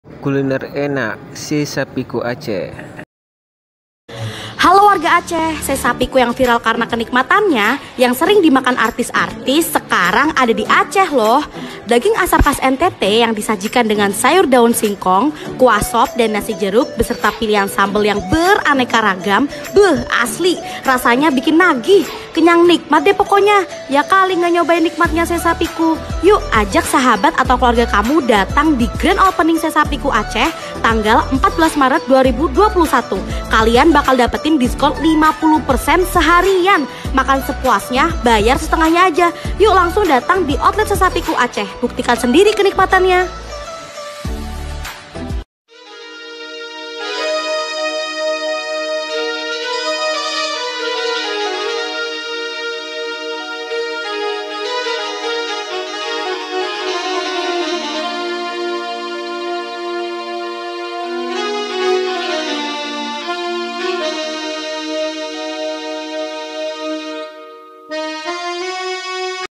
Kuliner Enak Sei Sapiku Aceh
Salah satu kuliner enak adalah menu Sei Sapiku Aceh.

Kuliner ini dibuka sejak tanggal 14 Maret 2021. Lokasi di kawasan Ulee Lheue Banda Aceh.

Bila dari kota Banda Aceh, lokasi disebelah kiri jalan sebelum kuburan massal tsunami di Ulee Lheue Banda Aceh.